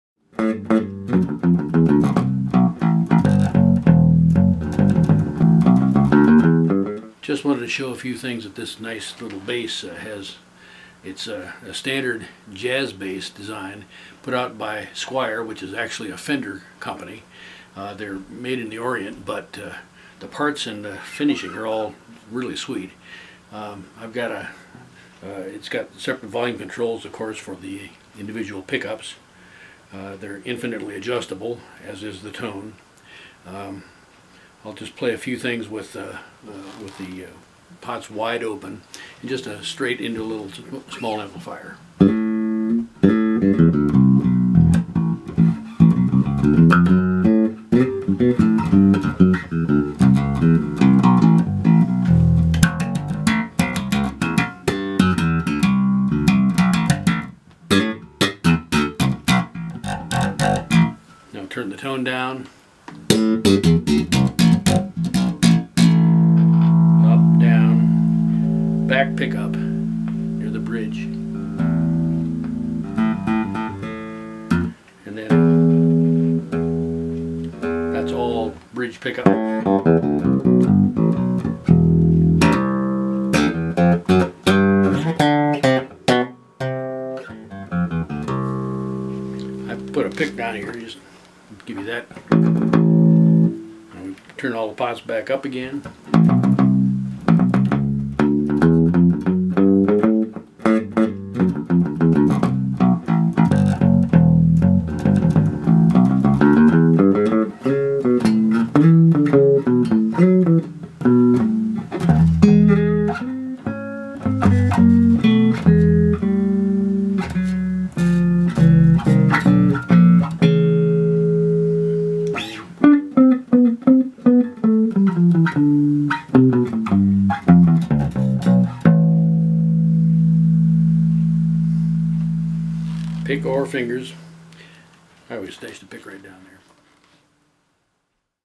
Squier Jazz Bass Demo
This easy to play 5 year old jazz/rock bass is set up with new English RotoSound "Swing Bass" strings.  Had them in my drawer for about 2 decades.  They really DO blow the doors off with clarity and brilliance.  With the tone turned down, they are fat, sweet and rich.  The intonation on the bass is perfect all the way up to the highest frets on every string.  Check it out...